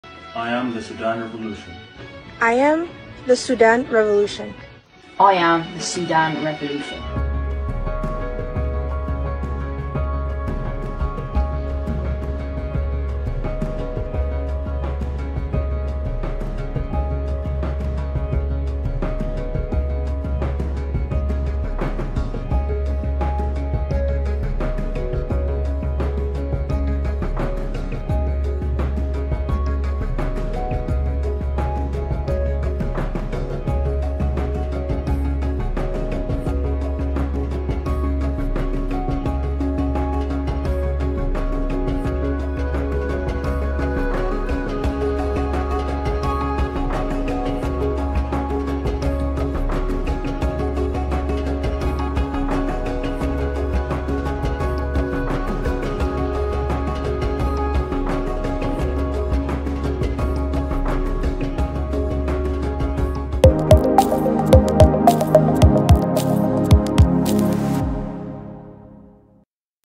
Why profil pictures are blue ? 💙
Since Sudan crisis, social media users expressed their support... 🇸🇩 🙏🏾